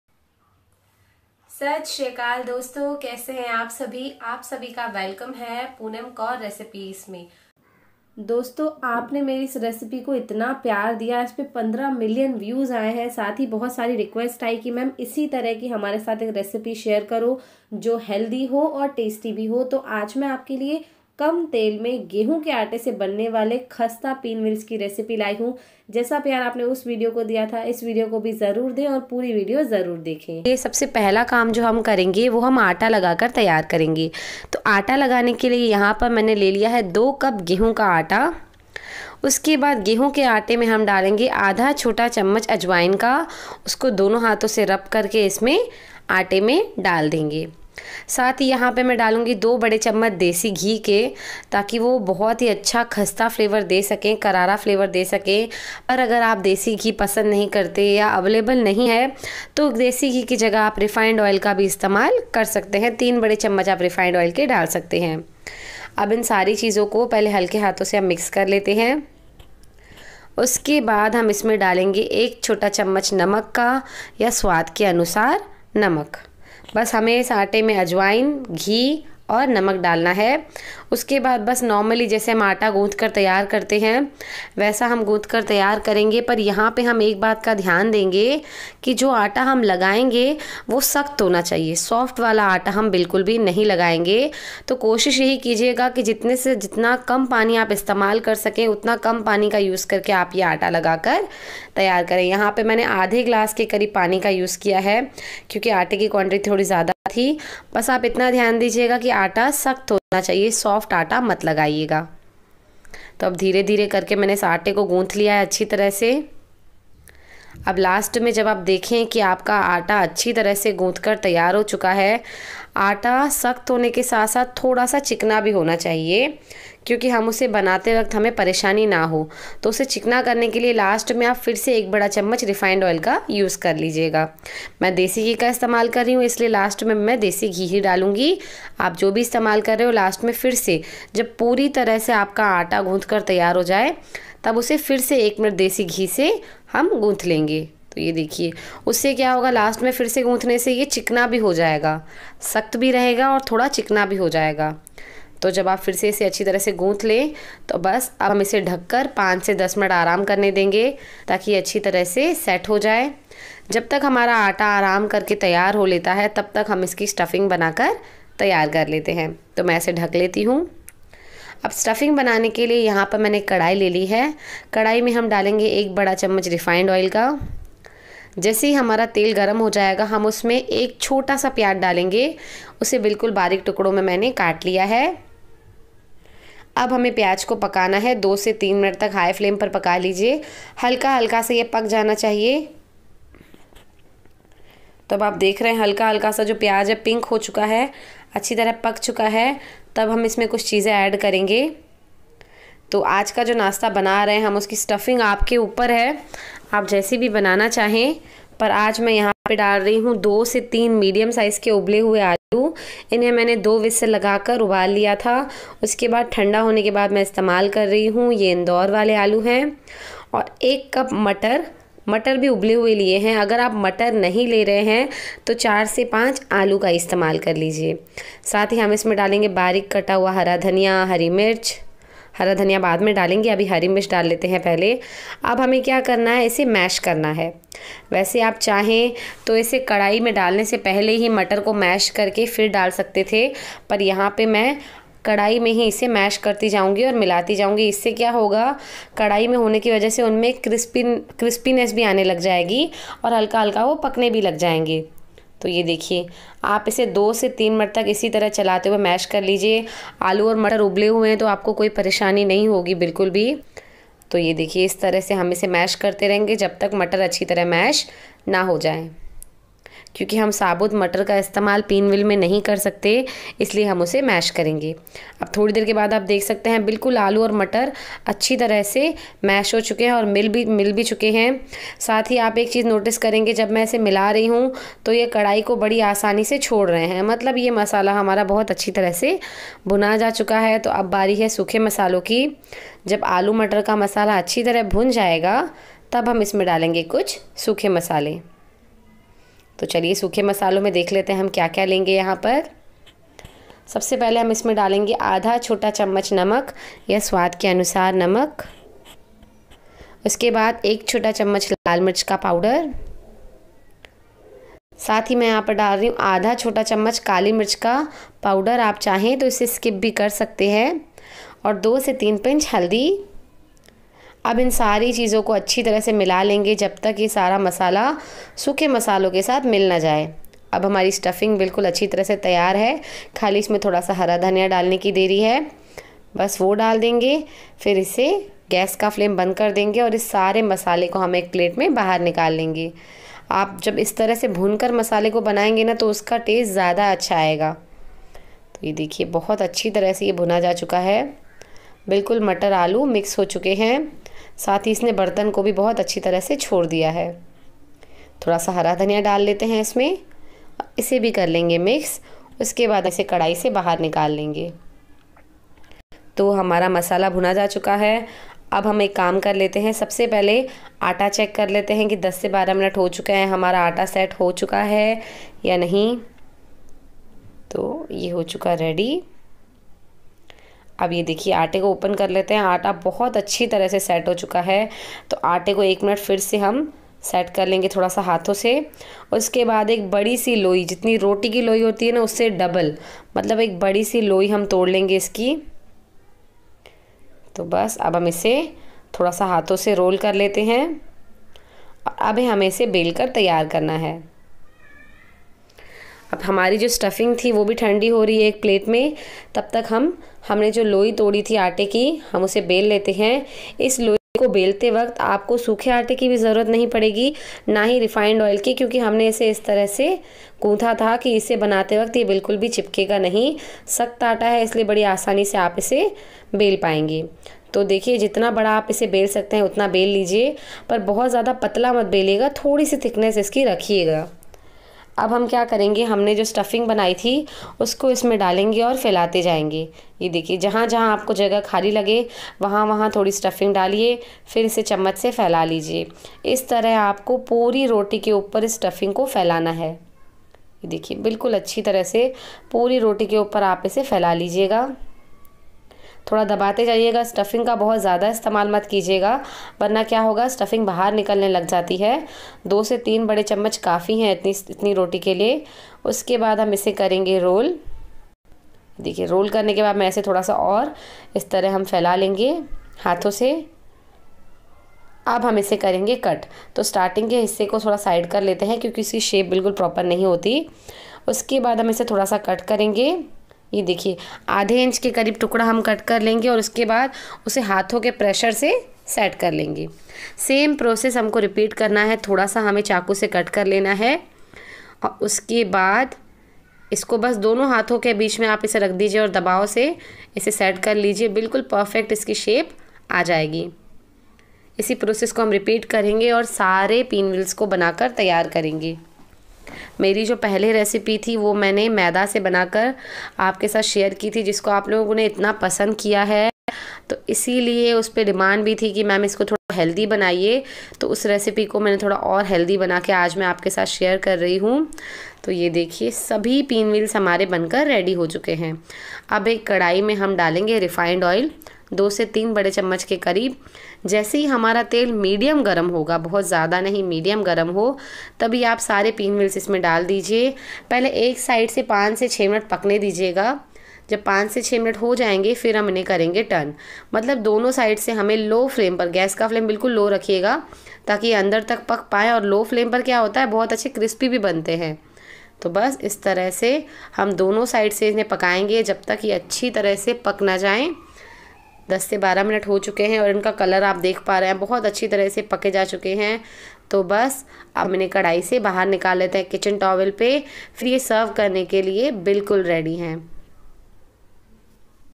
Potato Pinwheel Recipe|समोसा पिनवील्स वो भी बिना तले और गेहूं के आटे से|Aloo Bhakarwadi For tiffin
How to make patatoes pinwheel
घर पर बडी आसानी से समोसा पिनवील्स
बनाए